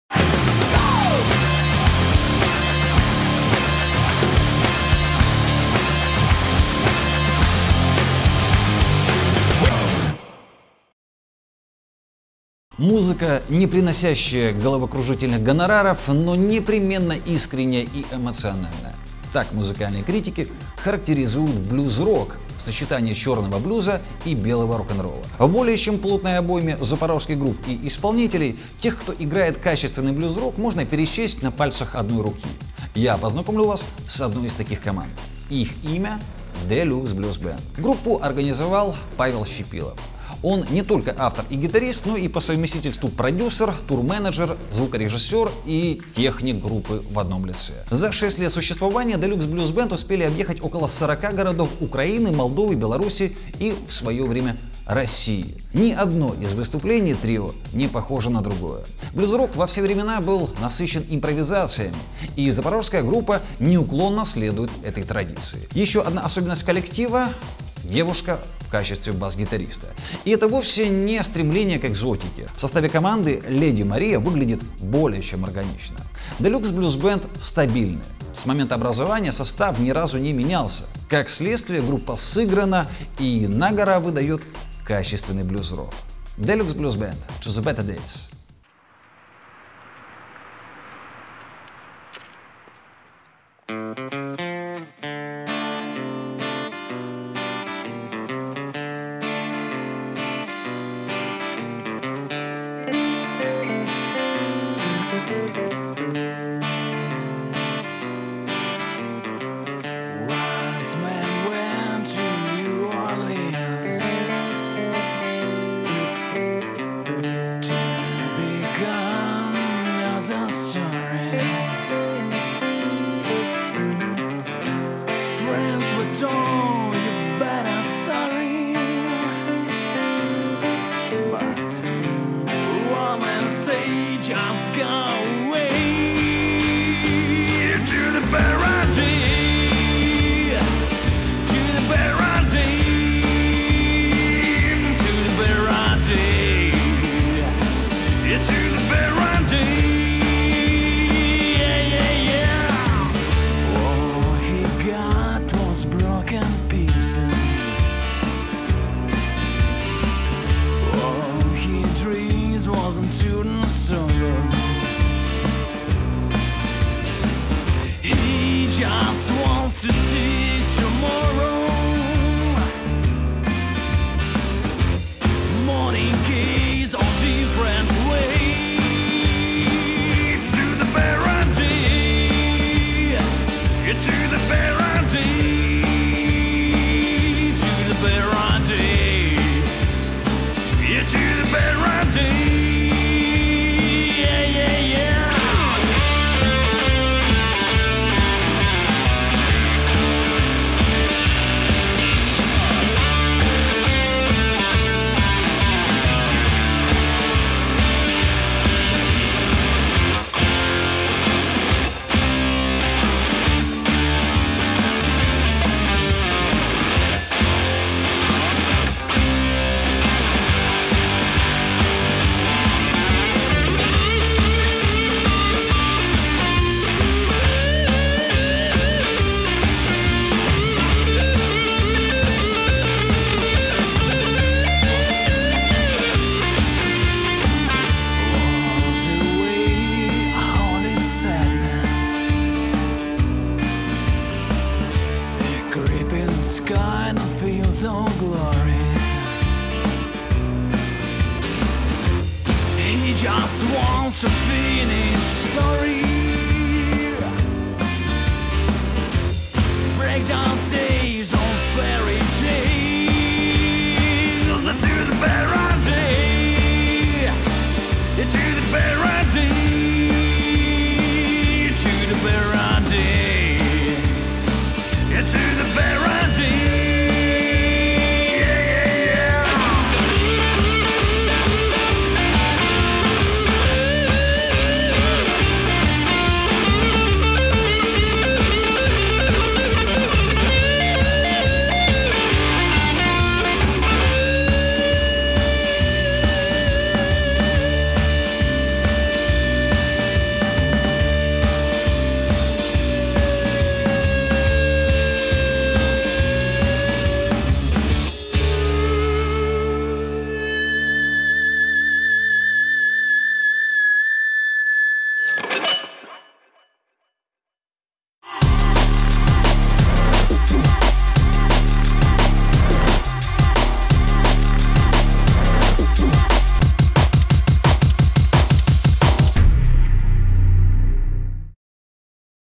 ZP Music Deluxe Blues Band
Выпуск о запорожской блюзовой команде Deluxe Blues Band.